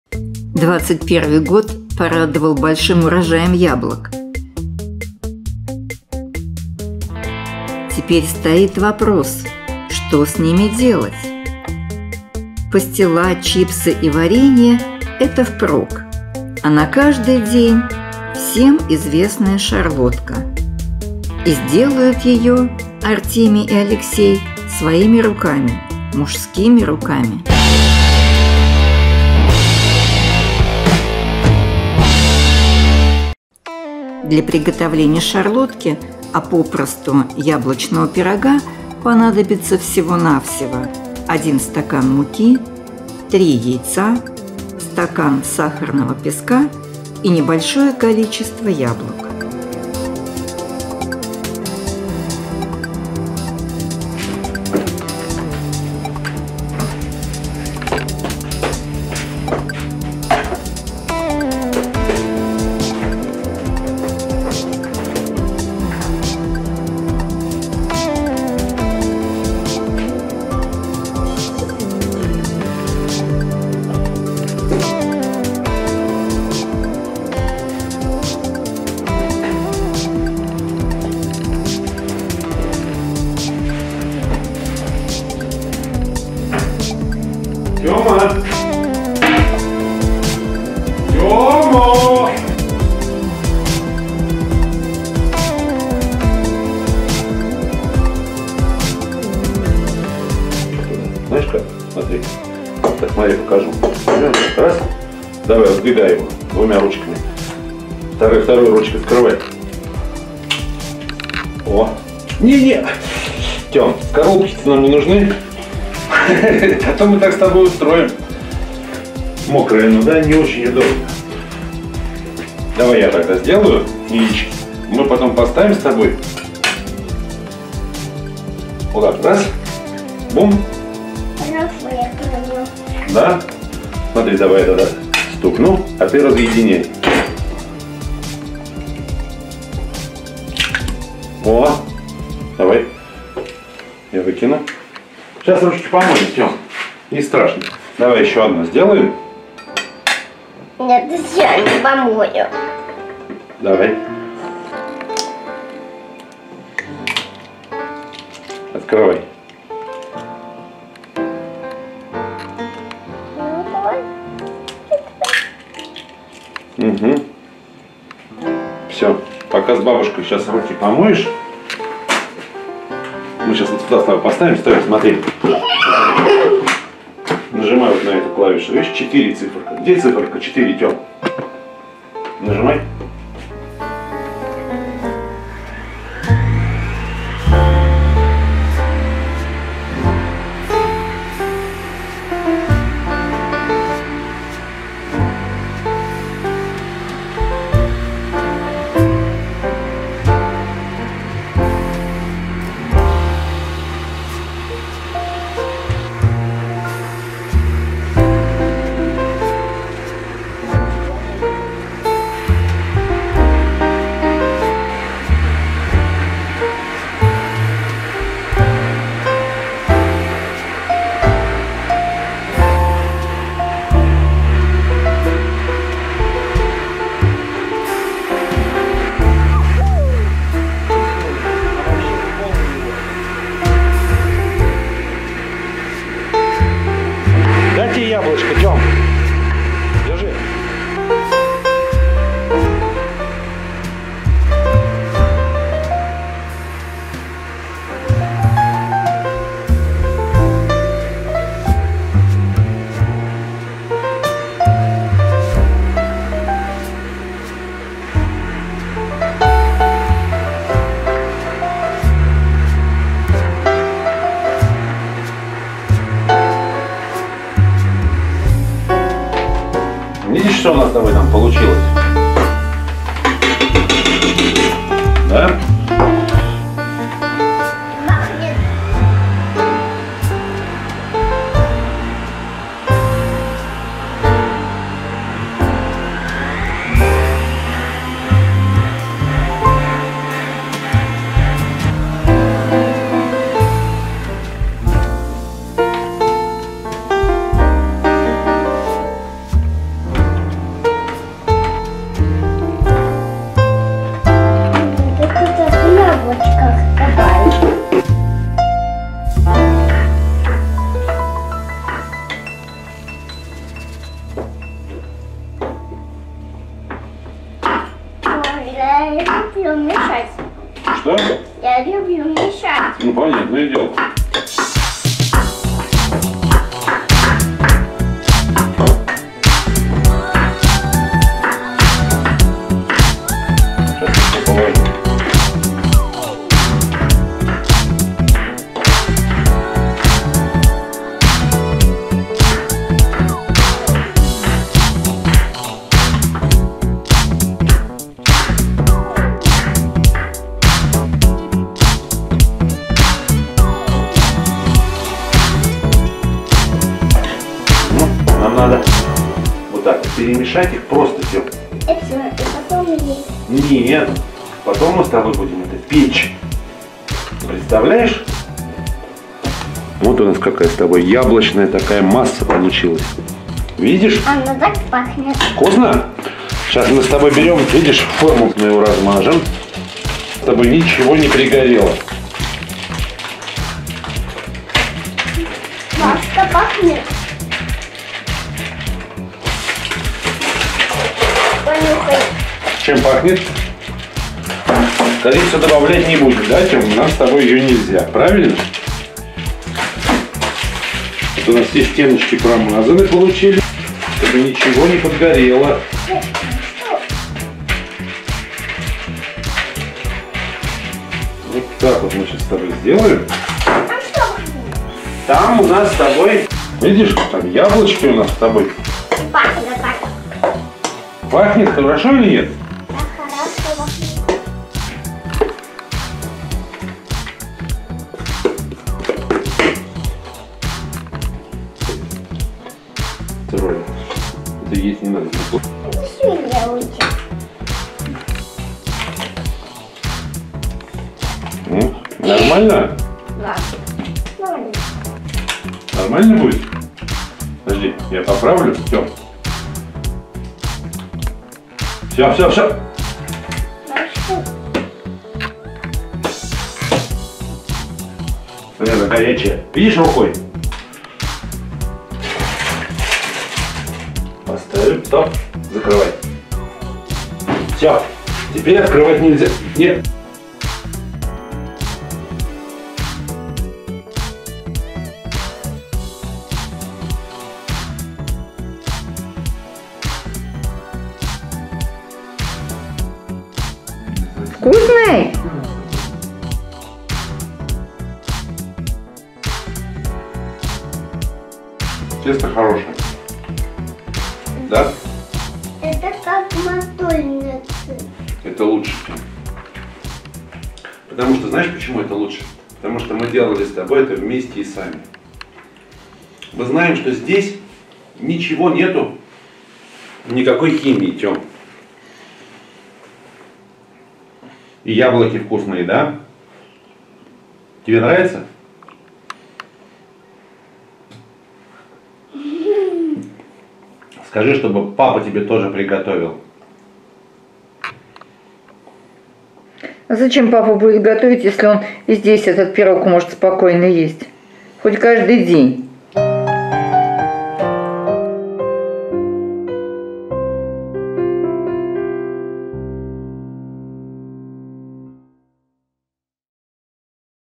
Шарлотка от Артемия мужскими руками. 28 .09. 2021
Яблочный пирог или "Шарлотка" известен всем. Уникальность нашего яблочного пирога состоит в том, что он приготовлен Артемием, которому в этот момент три с половиной года.